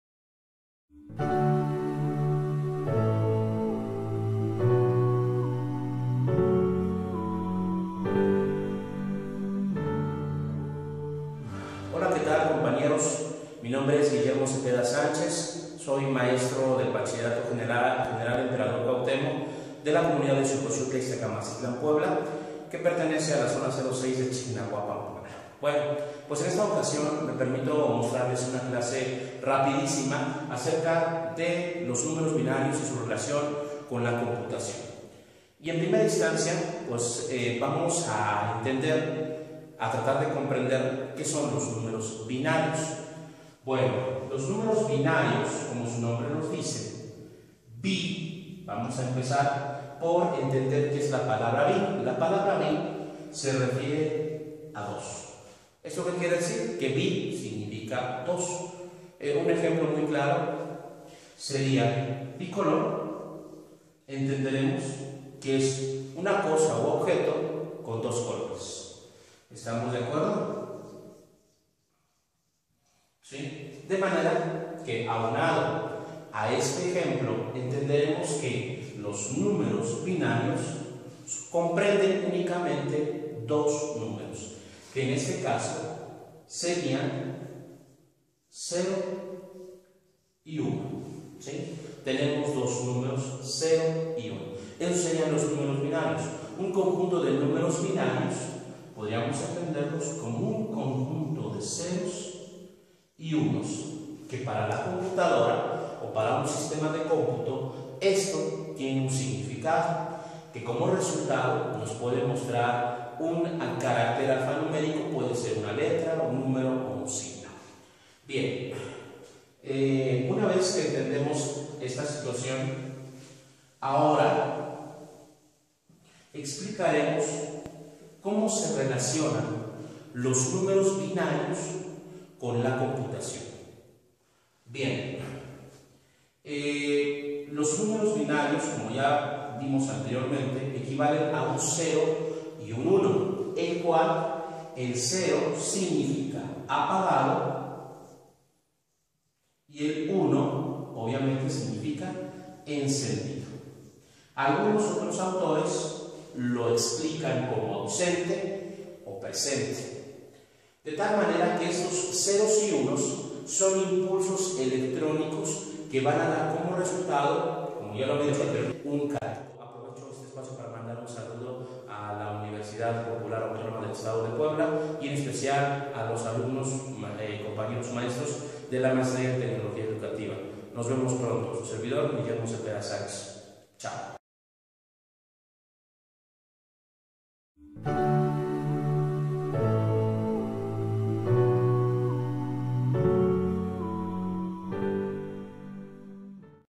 CLASE NUMEROS BINARIOS 2
En éste vídeo, se explica de manera general, el papel que juega la numeración binaria y su relación con la computación...